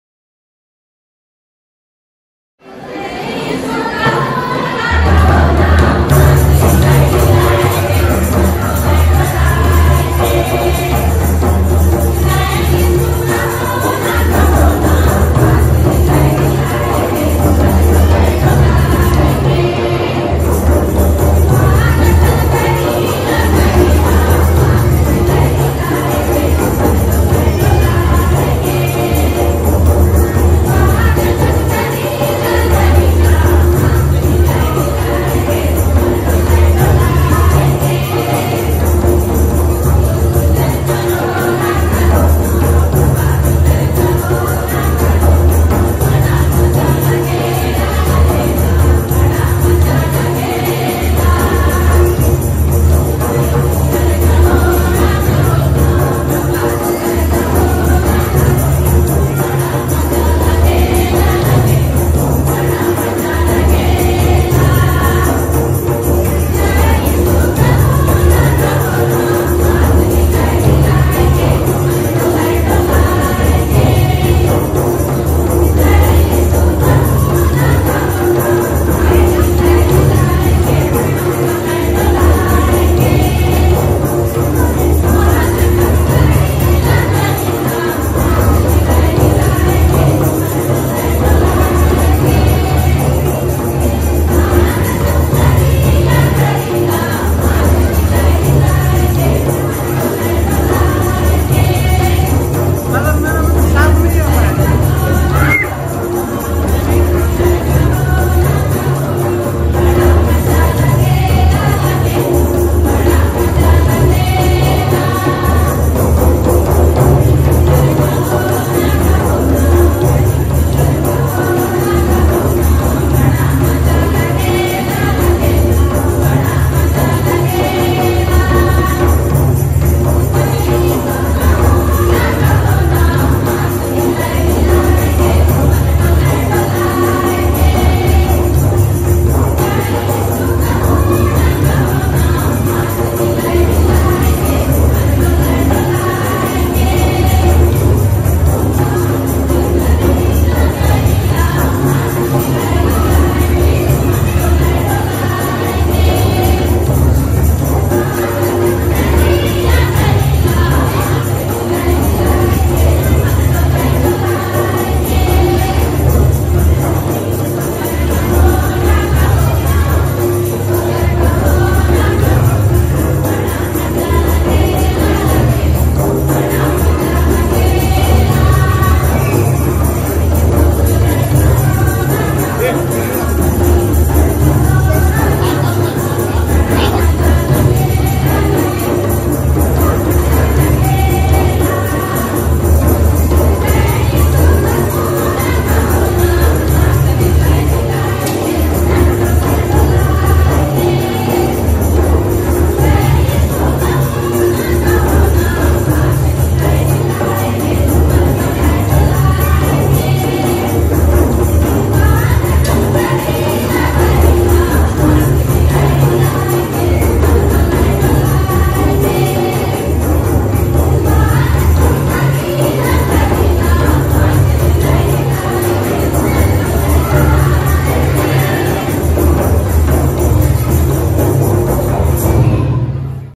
Family Day Celebration 2023, St. Theresa Bandra (W)
Performed by SAHARA Self Help Group, Bandra (W)
This video is not intended to generate any revenue and is only for documentation purposes…